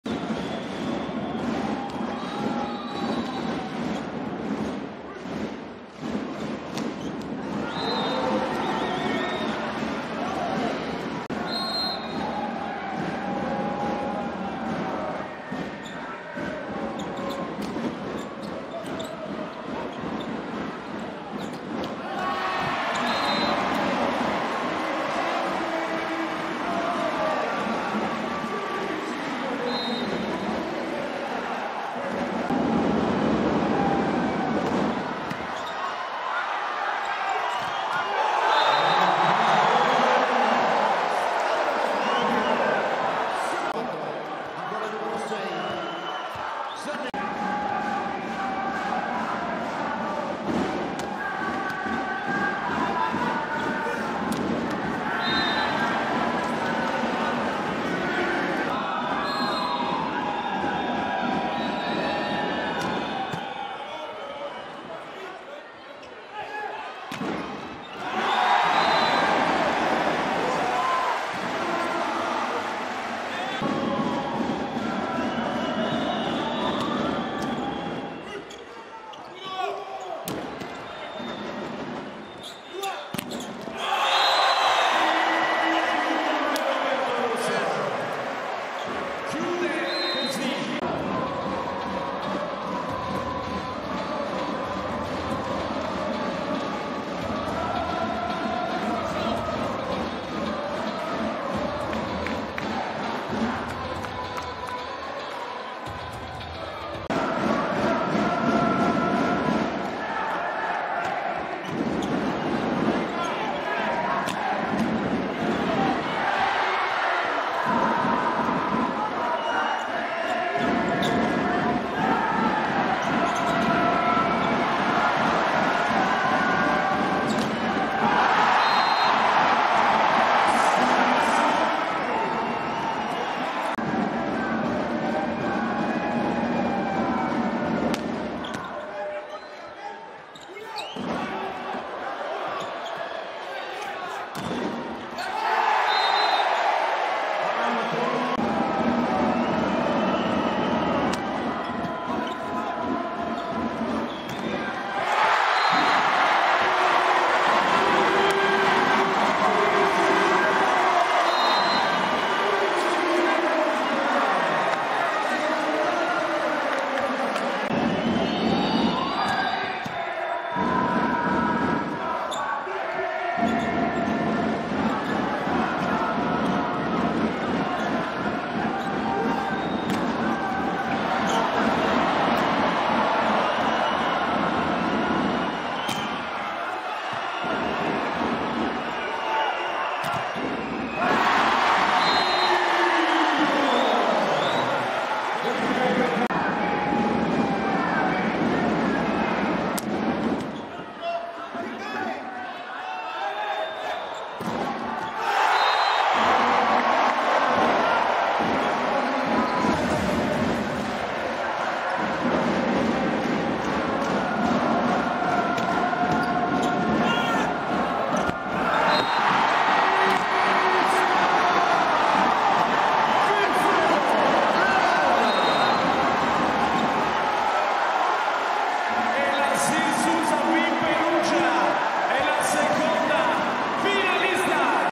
Sir Susa Vim Perugia vs Allianz Milano - Semifinale Del Monte Coppa Italia
Rivivi i momenti salienti della semifinale Del Monte Coppa Italia tra Milano e Perugia!